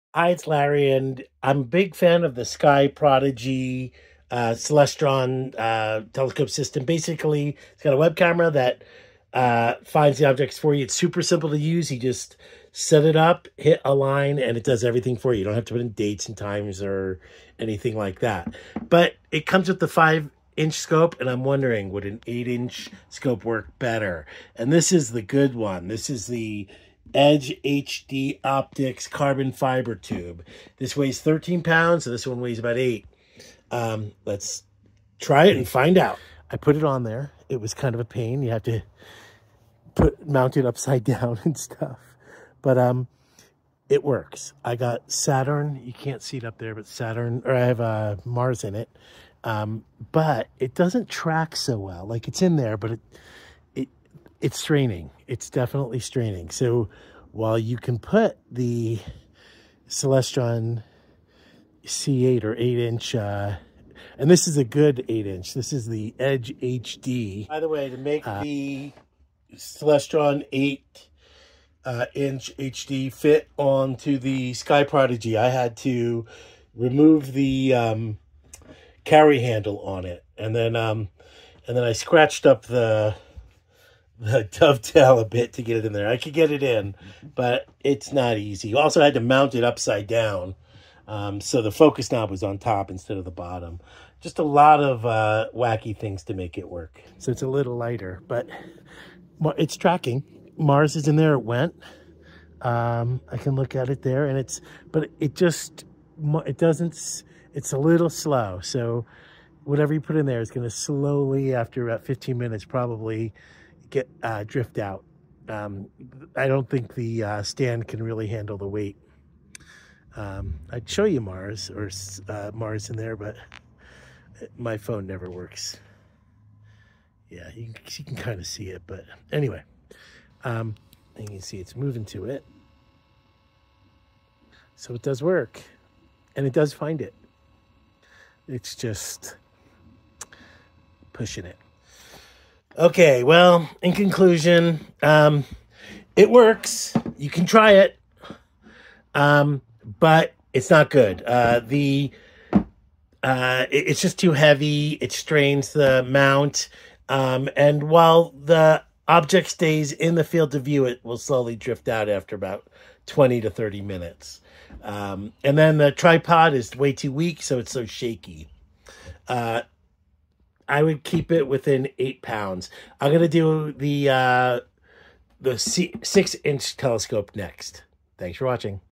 Mounting Celestron 8" Edge HD on SkyProdigy Starsense Mount, Will it work?
Tried mounting a Celestron Edge HD 8 inch (200mm) SCT telescope on the SkyProdigy StarSense mount. While it worked I doubt it would work for long. It was slow, and made straining noises and the object would drift out of the field of view after 10-20 minutes.